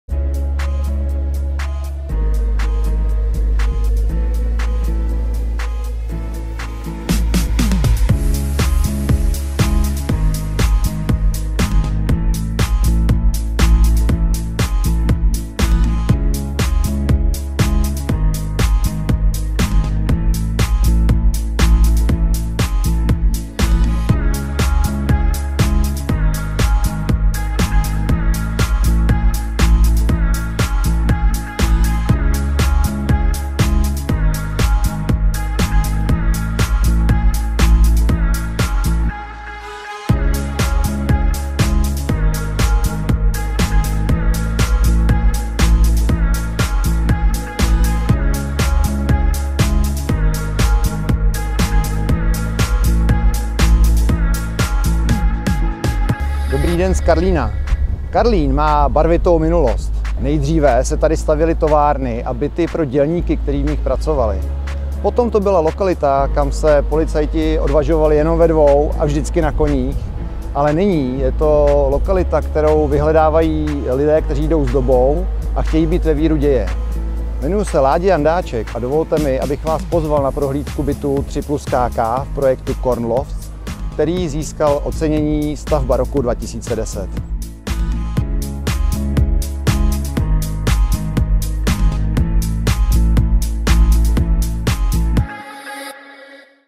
Prodej 3+kk Šaldova, Karlín
V Zastoupení majitele Vám nabízím k prodeji byt 3+kk s podlahovou plochou 94m2. K dispozici jsou dvě prostorné lodžie, koupelna, dvě toalety a komora.
Vstupními dveřmi se přes zádveří dostanete do obývacího pokoje spojeného s kuchyní a z obou částí lze vyjít na lodžii. Je opatřena skleněnou zástěnou s potiskem, takže umožňuje regulovat míru intimity na balkóně i v obytných místnostech. Kuchyňská linka s vestavnými spotřebiči nabízí téměř tři metry pracovní plochy. V horní koupelně najdete vanu se skleněnou zástěnou, umyvadlem a toaletou.
Schody se dostanete do spodního patra, kde se nachází koupelna s umyvadlem a 2 místnosti, které nyní slouží jako ložnice a pokoj pro hosty. Ložnice má velkorysých 20 m2, druhý pokoj 16 m2 a je zde k dispozici balkón. Prostor pod schody je využíván jako komora.
V celém bytě jsou položeny dřevěné podlahy a v koupelně najdete luxusní dlažbu s keramickým obkladem a dřevem.
Projekt Cornlofts byl dokončen v roce 2009 a vznikl přestavěním lisovny řepkového oleje z konce 19. století. Architekti umístili byty tak, aby každý z nich měl okna jak do ulice, kde jsou zachovány původní prvky stavby, tak i do vnitrobloku, kde jsou umístěny lodžie.
Díky tomu dokázali snoubit původní styl s tím moderním a tak splňuje požadavky i těch nejnáročnějších klientů.
Hledáte-li stylové bydlení v jedné z nejatraktivnějších lokalit v Praze, blízko centra, je tato nabídka přesně pro Vás. Těším se na společnou prohlídku tohoto krásného a přepychového bytu. Ladislav Jandáček